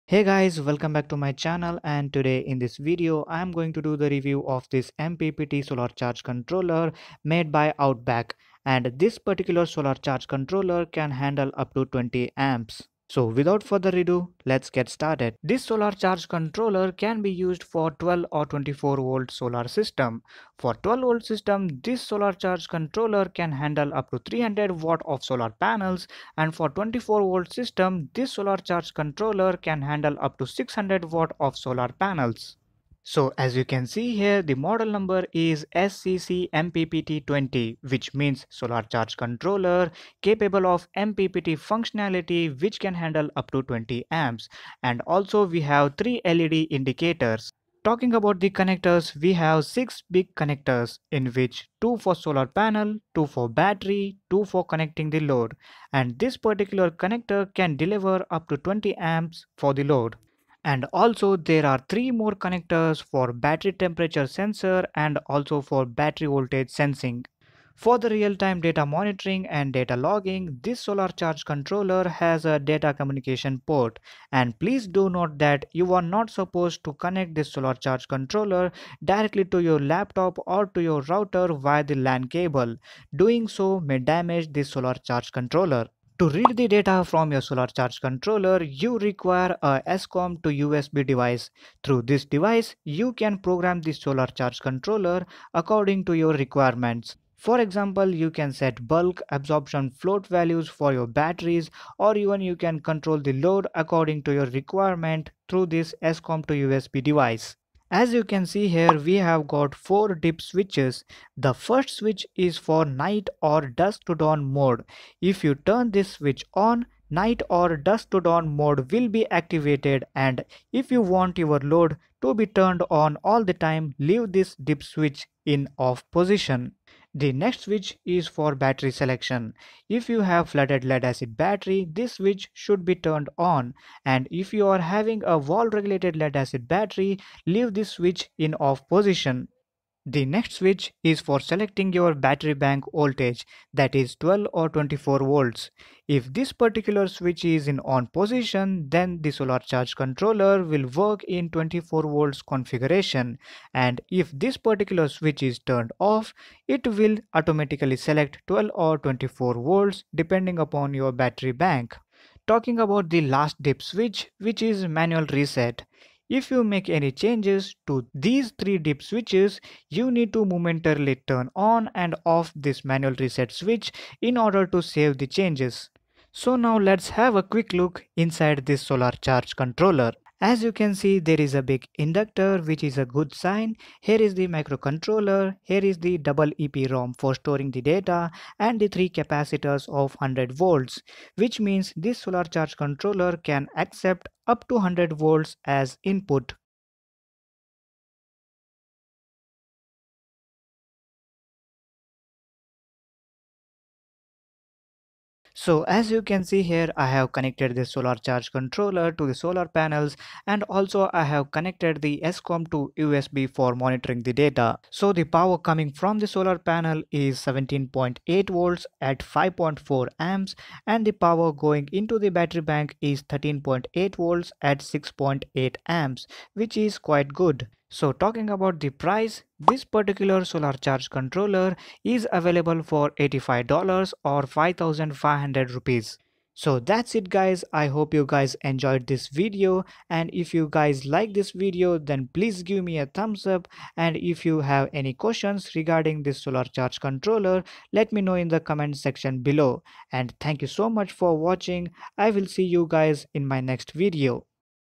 Best MPPT Charge Controllers (20A | 12/24v) by Outback power
Hey guys in this video i am going to review this solar charge controller  made by Outback power.

This solar charge controller can be used for 12 or 24 v solar system with input of 100v DC.